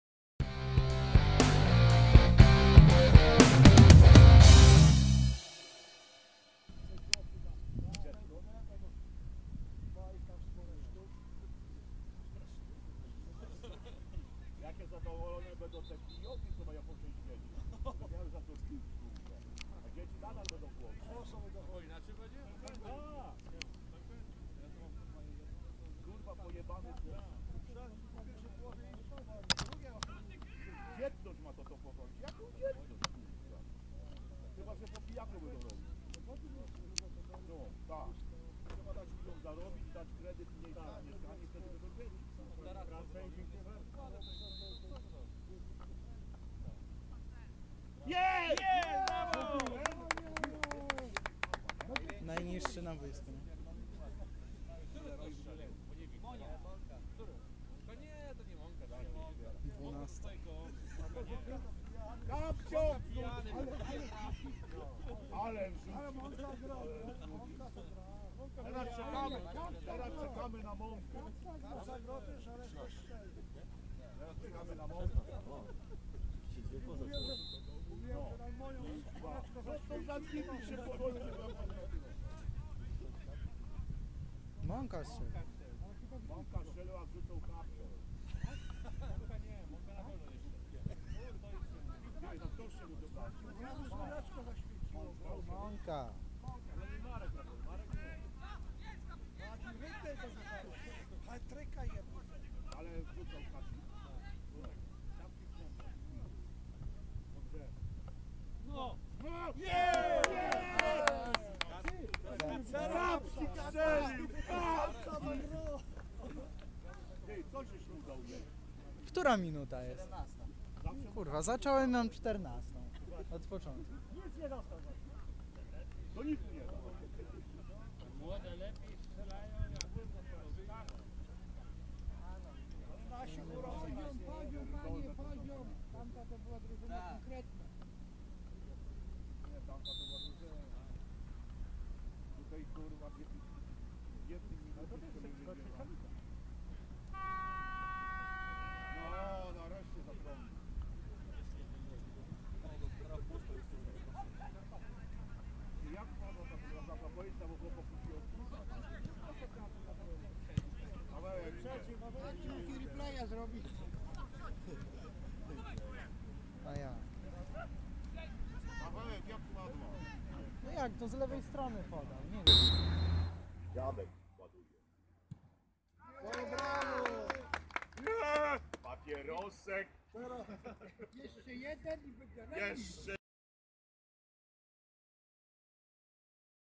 Sparing: KSZO Ostrowiec Św. - JKS Jarosław 4:2 (3:1)
Sparing trzecioligowców KSZO - JKS Jarosław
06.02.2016 r. Ostrowiec Św. Piłkarski Ośrodek Treningowy przy ul. Kolejowej